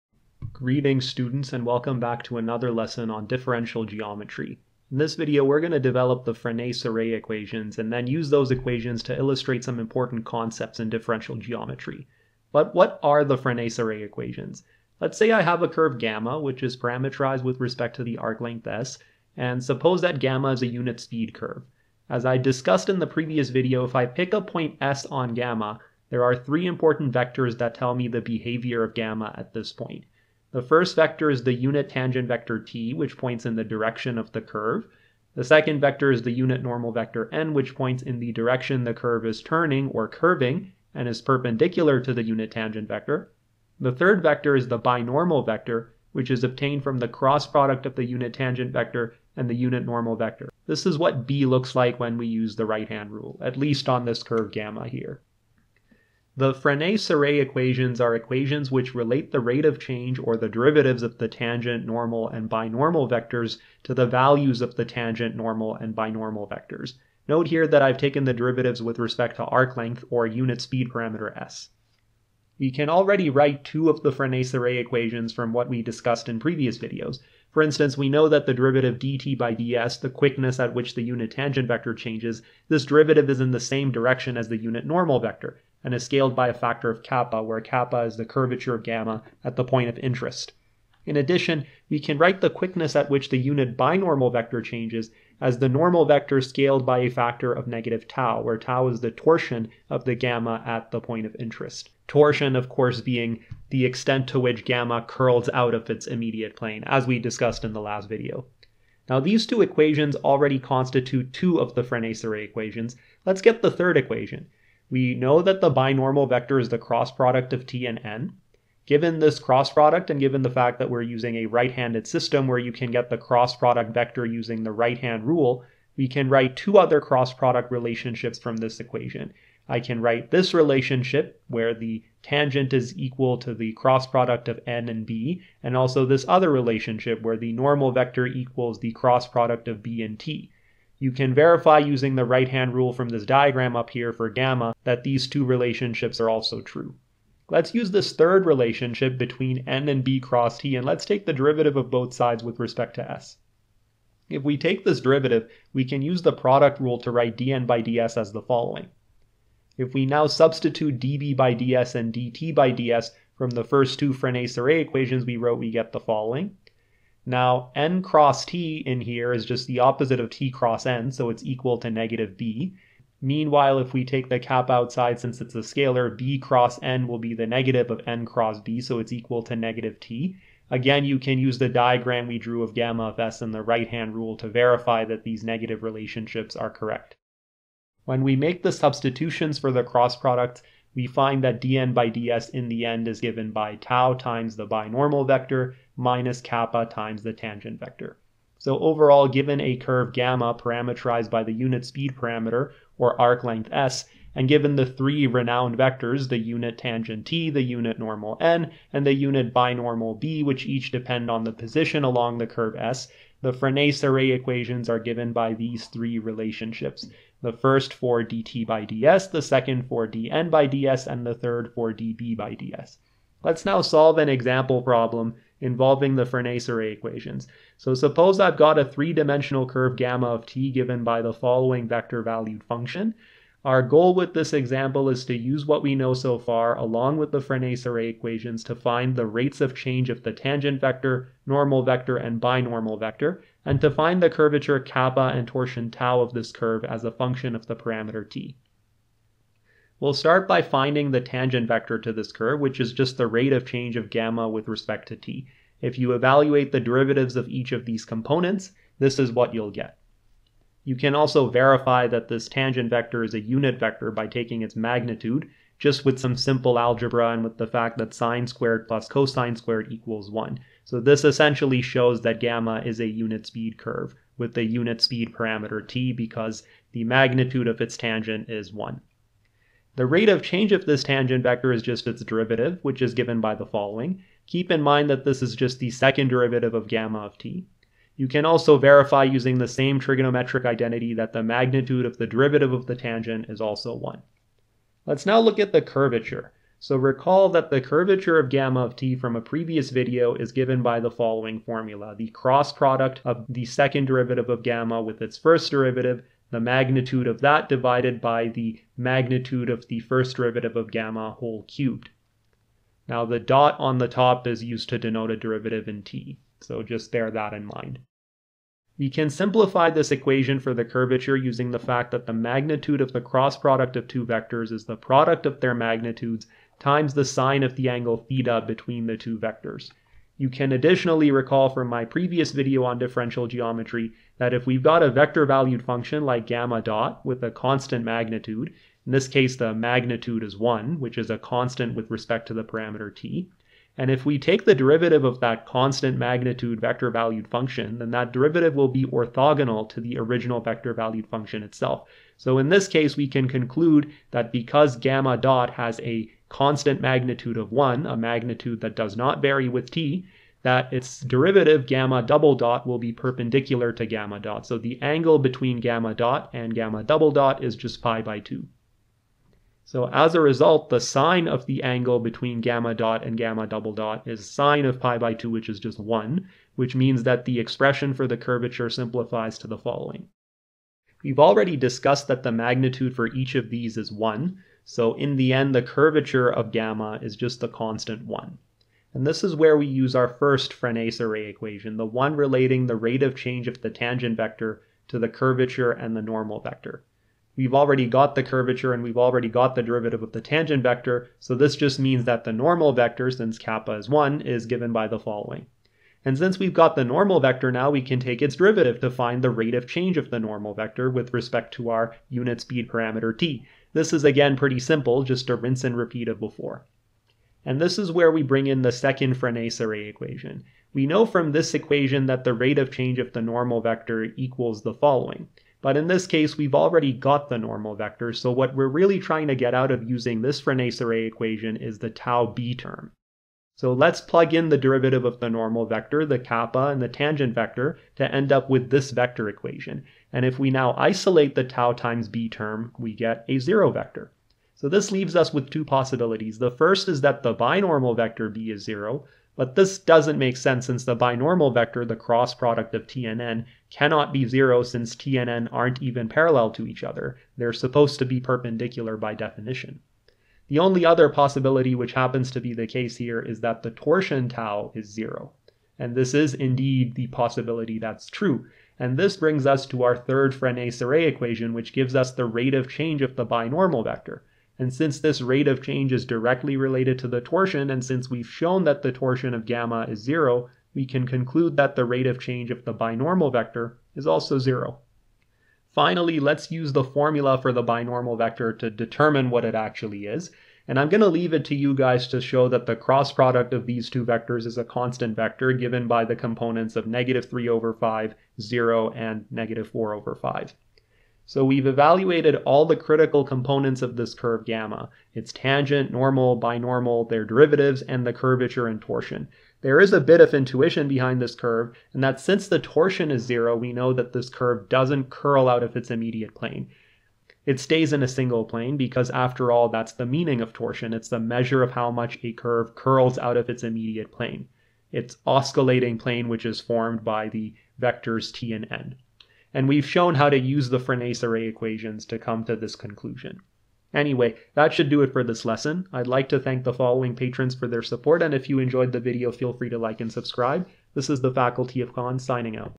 The Frenet-Serret Formulas | Differential Geometry
In this edition of DifferentialGeometry, I introduce and derive the FrenetSerret Equations/Formulas, and go through an example applying them to a space curve.

Special thanks to my Patrons:
Patapom
Eugene Bulkin
Andy Johnston
Lina Stritt
Patrick Gibson
Renato Lutz